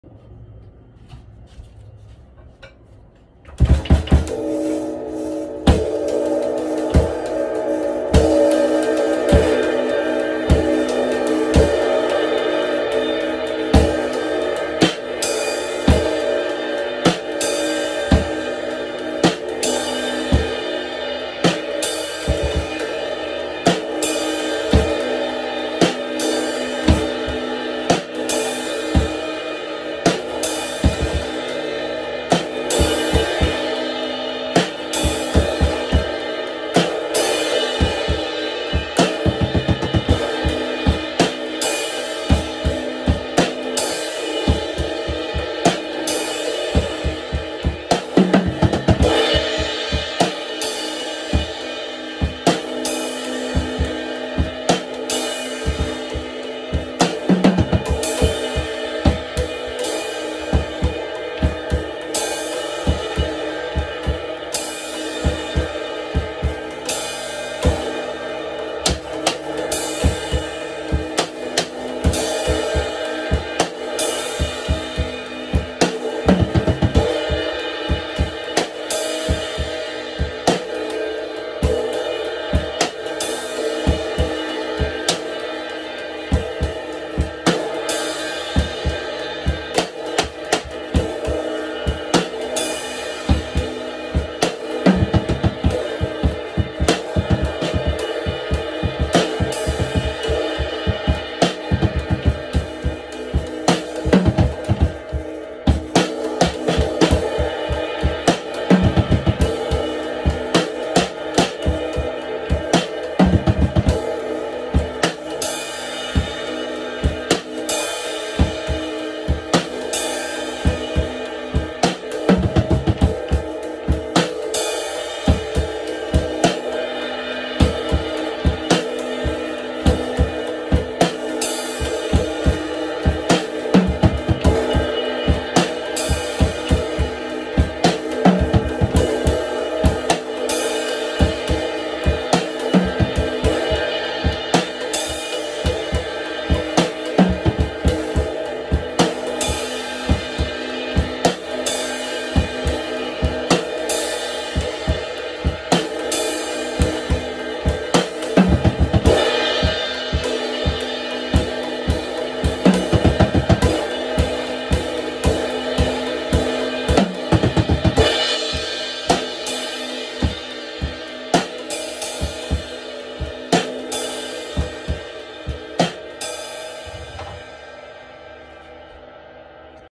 Paper towel roll tube mallet test
I tried to use the old inside of a paper towel roll as a soft mallet on the ride cymbal.